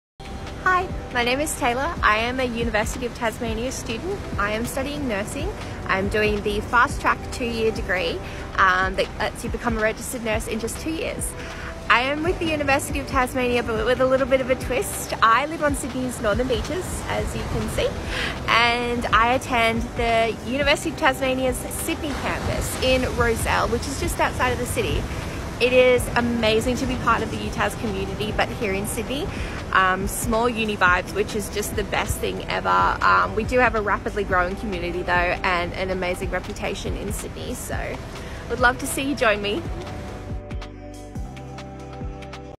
Small uni vibes...in Sydney?
Let Taylor tell you all about the 'small uni vibes' on our Sydney campus in Rozelle, and find out more